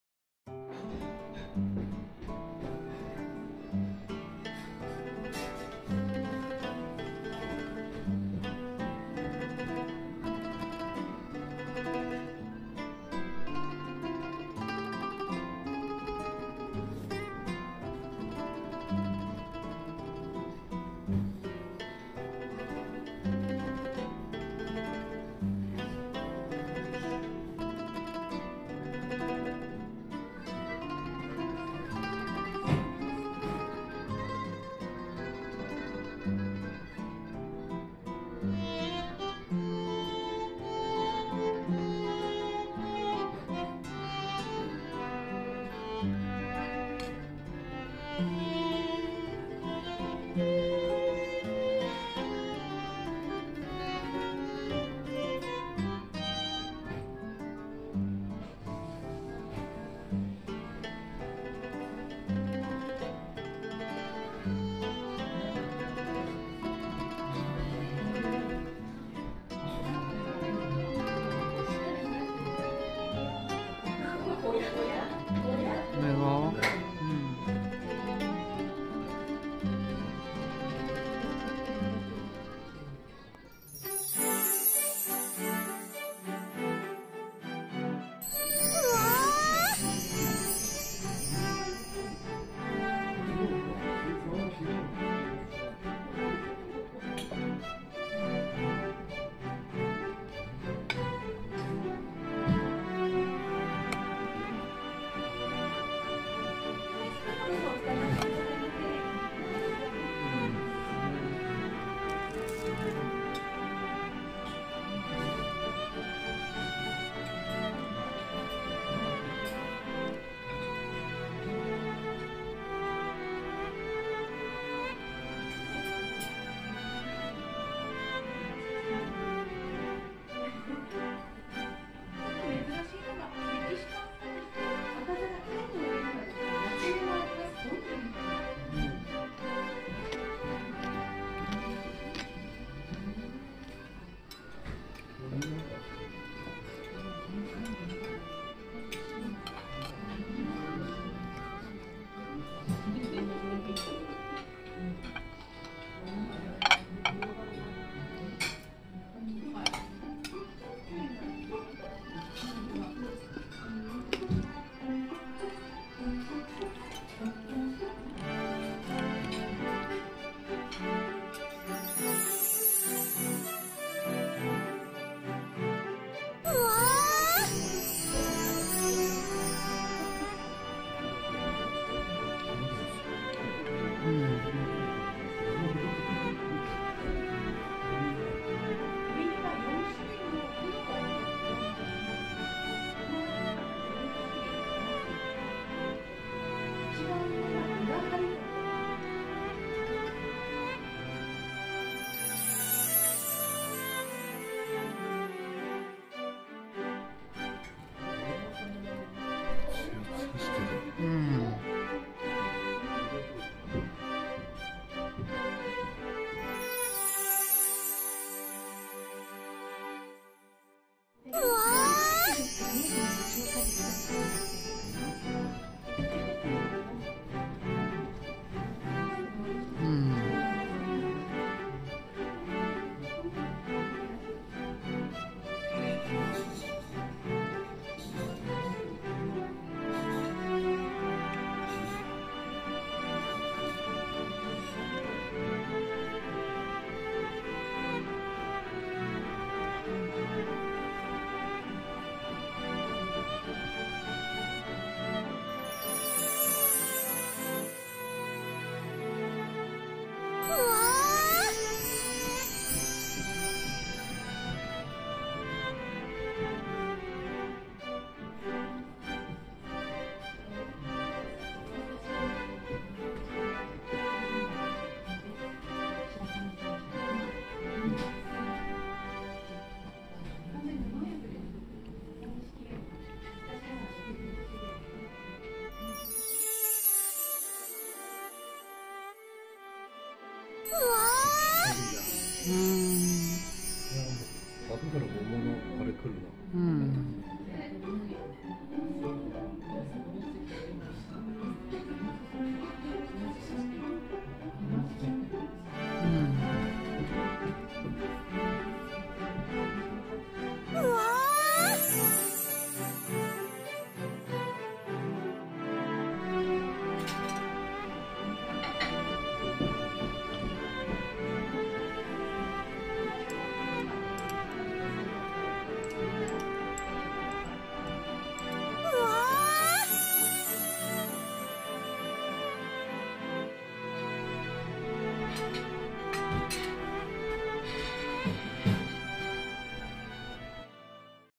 秋田県大仙市【ジュエーメ】最高なイタリアン！ 感動感激♡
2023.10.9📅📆
《秋田県大仙市 ジュエーメ》🇮🇹
※支払いは全ておばさんのお金で払っています。使った合計参考金額およそ13000円です。
ショート動画はこちら。